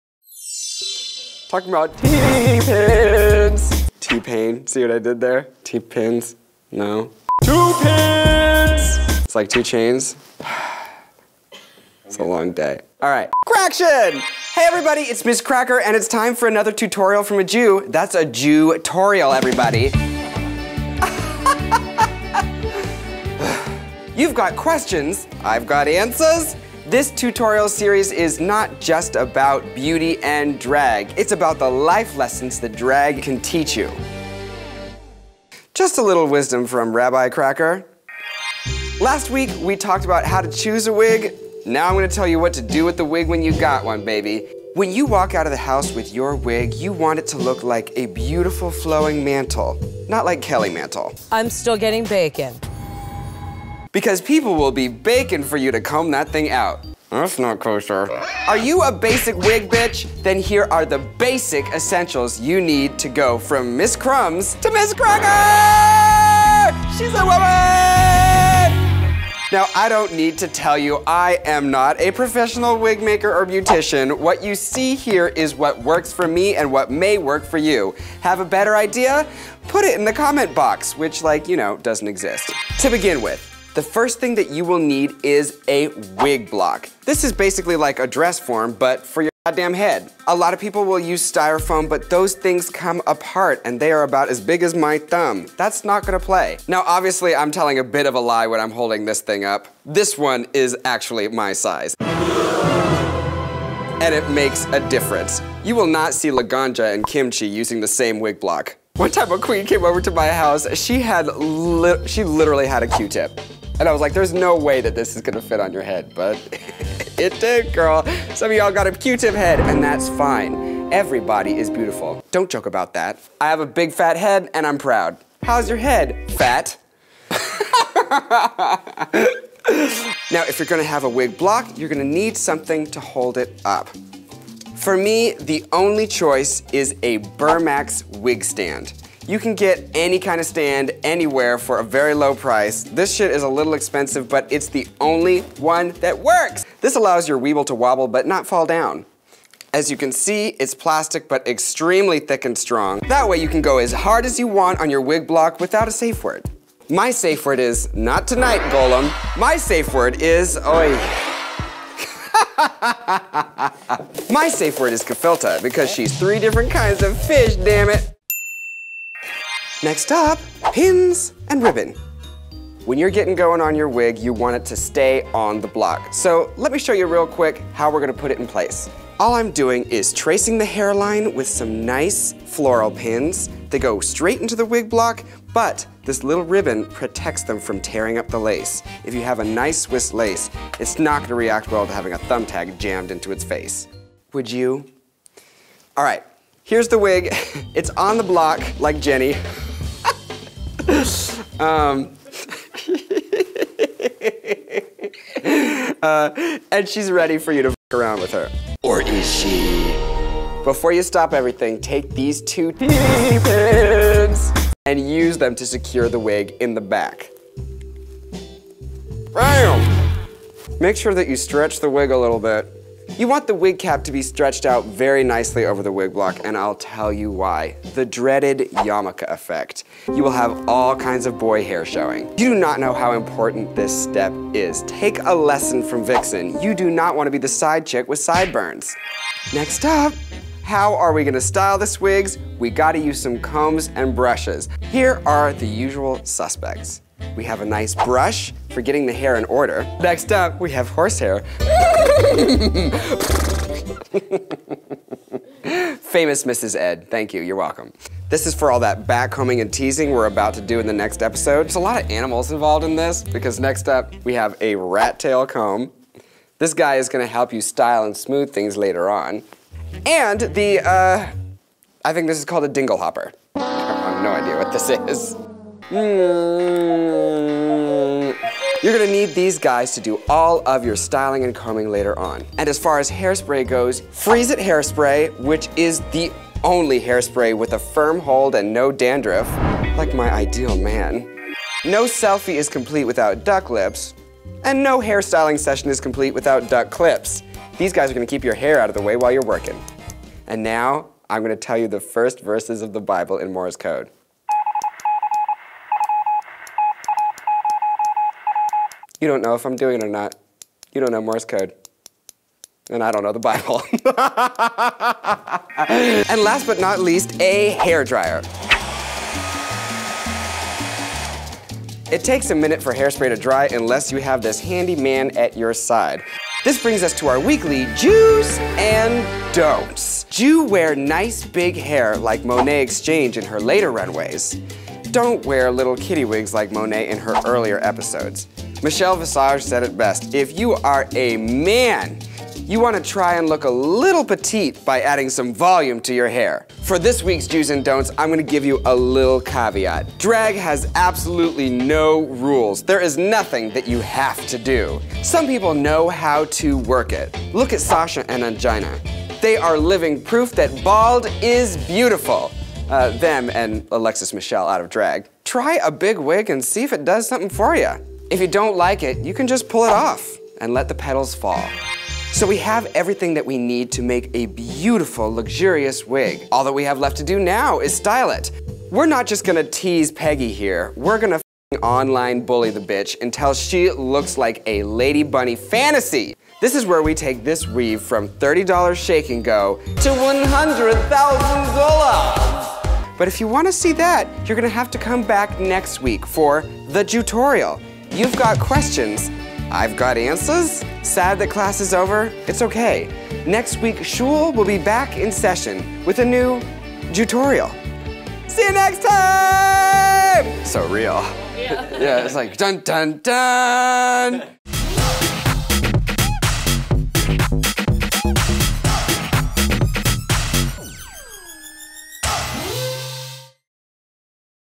What to Do With Your Wig: JewTorials with Miz Cracker
Miz Cracker of RuPaul's Drag Race season 10 teaches you how to prep your wig for styling!

Come to RuPaul's DragCon LA, May 24, 25, 26!